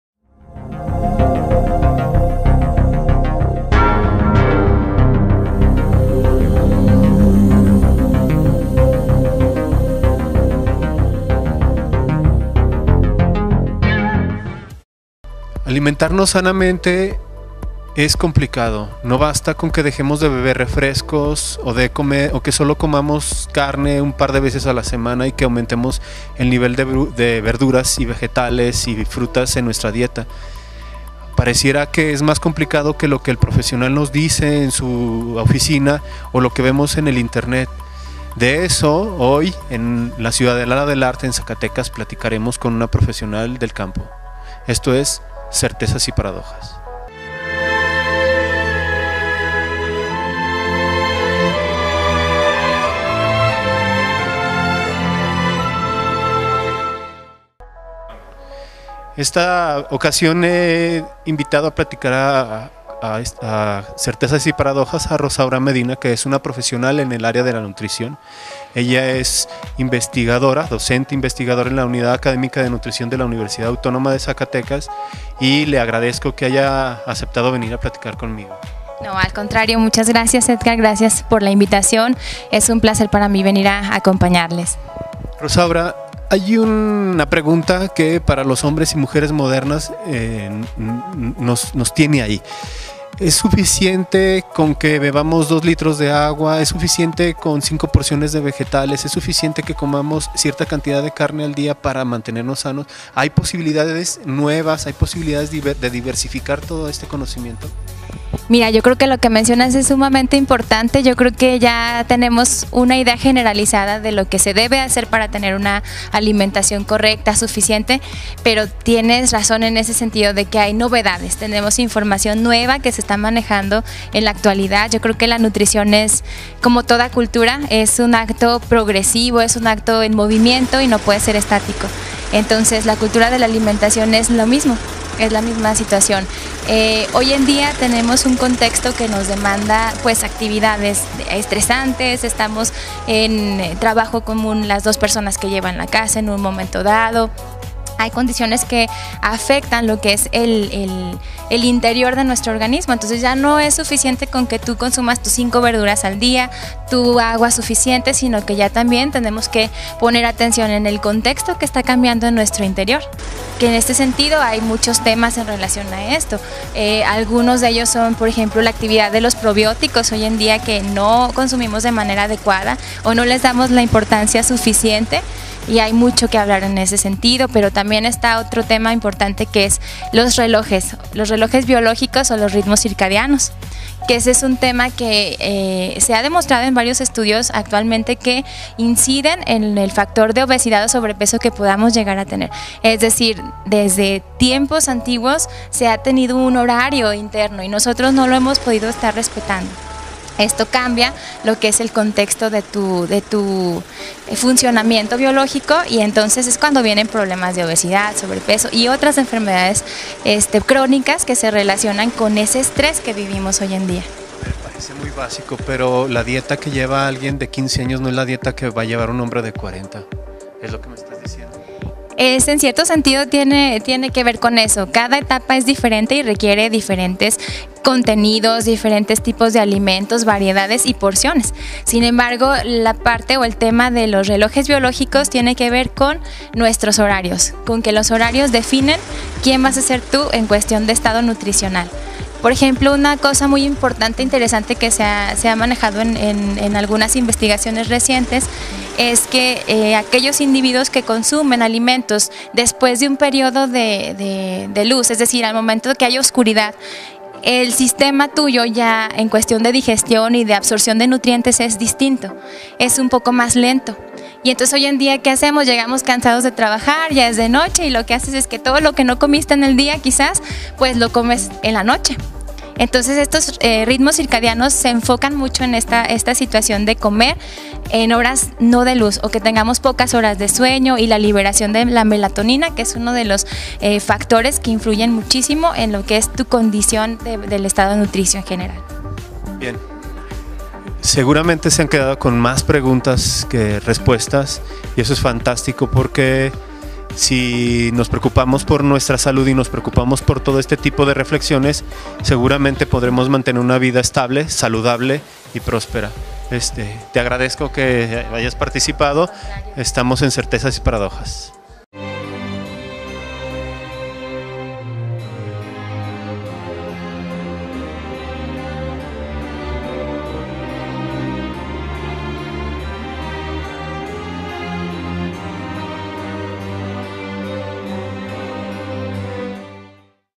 Certezas & Paradojas 2
Certezas & Paradojas 2 Capítulo 2: Entrevista con el Mtra. Rosaura Medina, docente de la Unidad Académica de Enfermería de la UAZ, quien habla de los fundamentos de una buena alimentación.